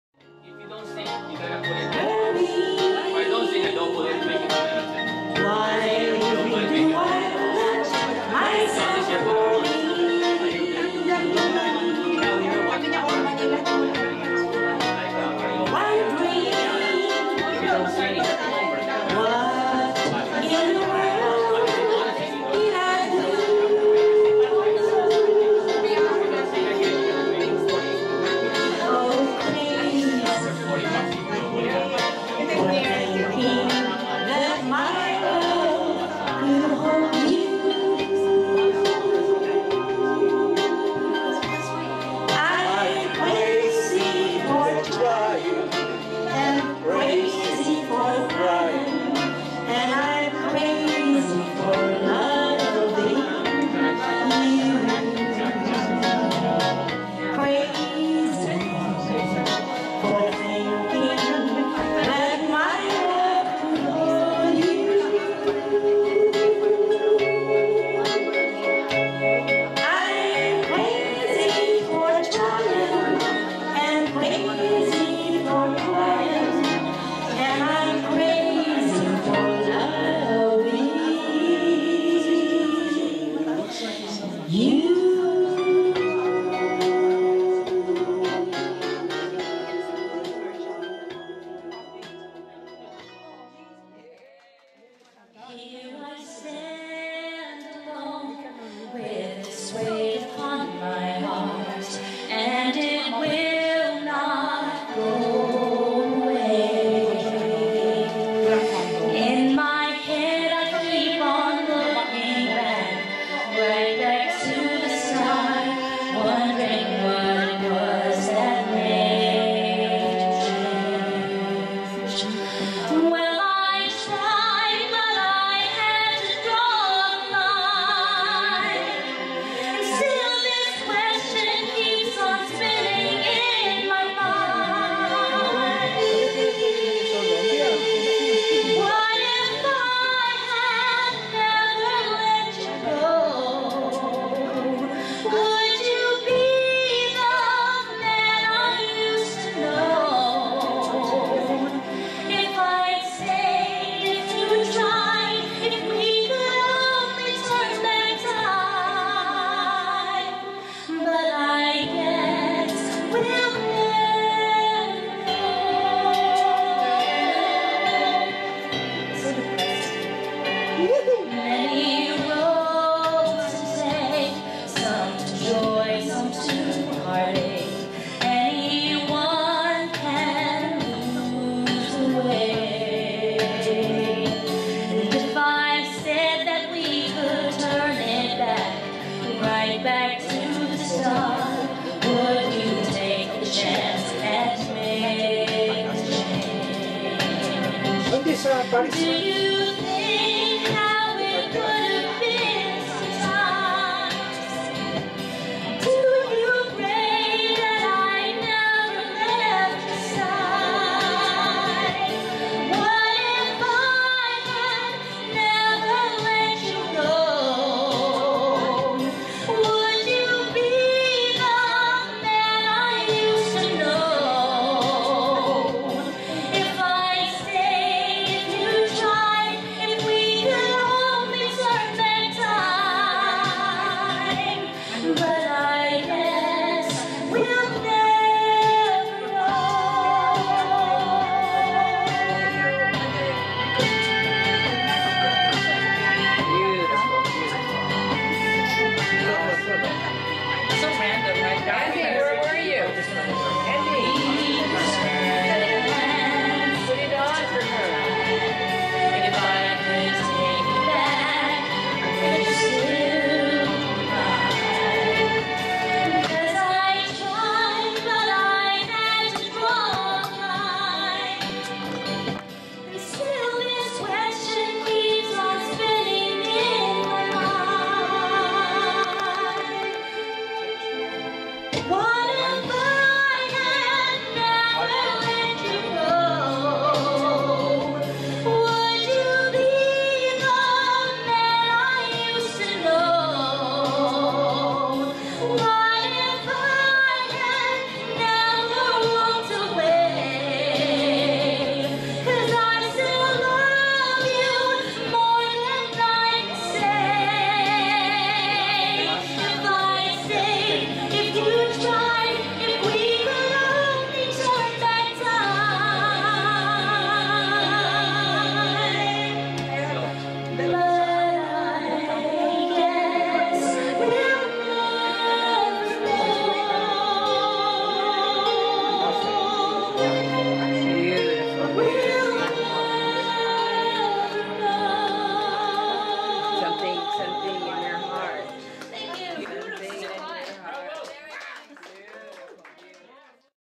140105 00620 Karaoke at Sol
Karaoke Party @ Sol - San Mateo CA on 2014-JAN-05